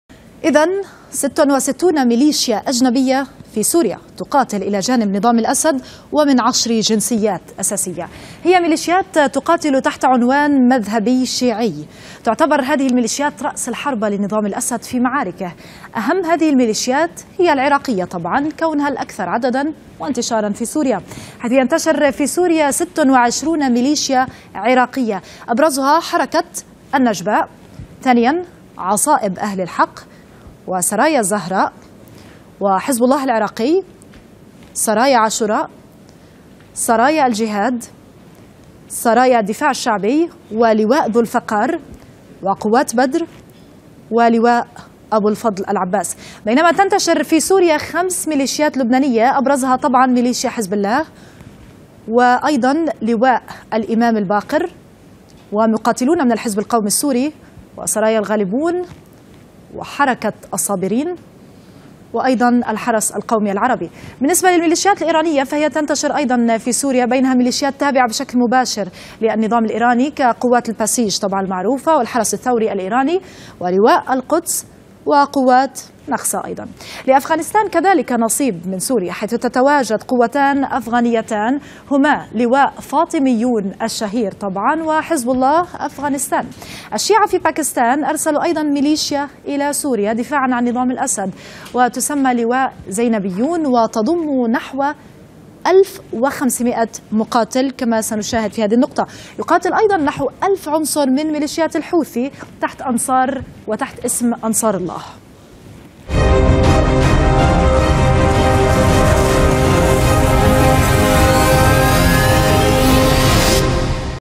66 ميليشيا طائفية تقاتل مع نظام الأسد في سوريا
66 ميليشيا تقاتل مع نظام الأسد في سوريا